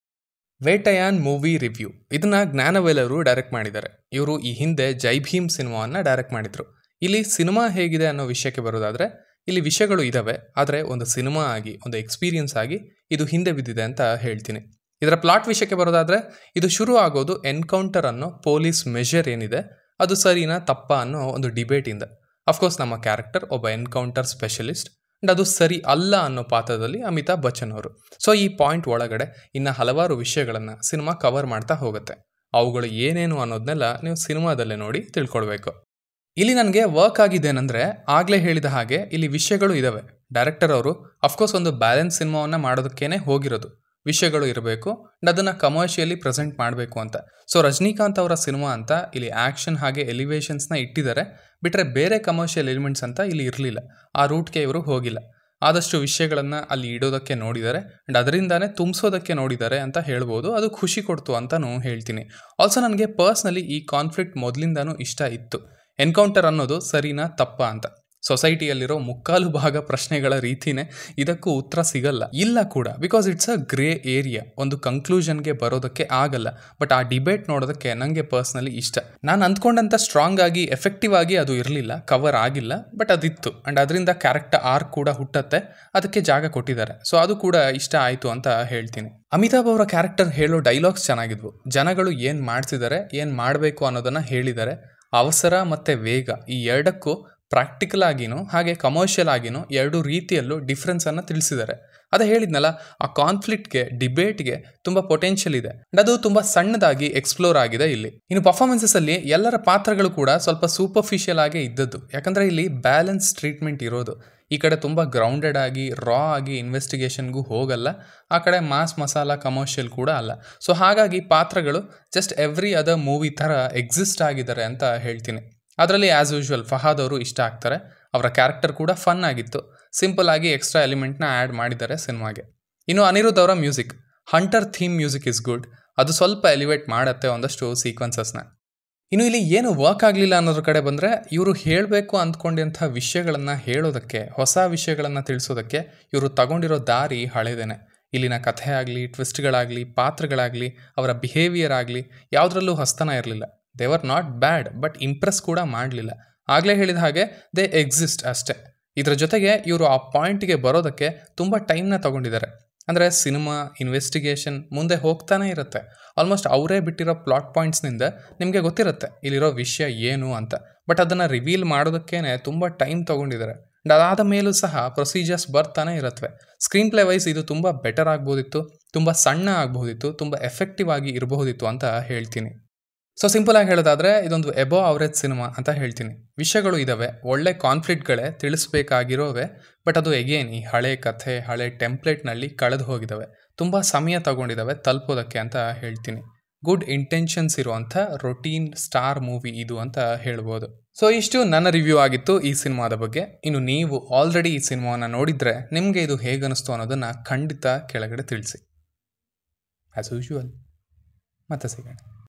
ಸಿನಿಮಾ ಹೇಗಿದೆ ಅಂದ್ರೆ.?🤔 | VETTAIYAN Movie Review in Kannada | Cinema with Varun
This is Just My Honest Opinion about the Movie.)
Above Average.)

VETTAIYAN Movie Review in Kannada.